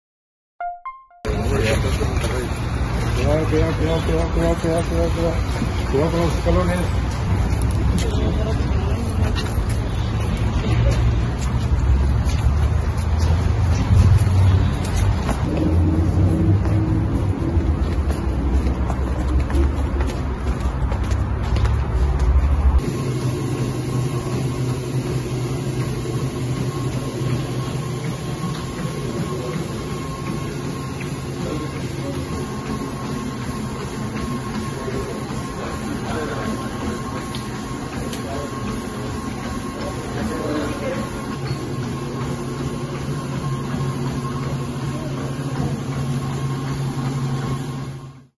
Jesús Angulo pasa las pruebas médicas con Tigres
El refuerzo de Tigres JesúsAngulo libró con éxito los exámenes físicos y este martes se incorporará de lleno al trabajo con el equipo.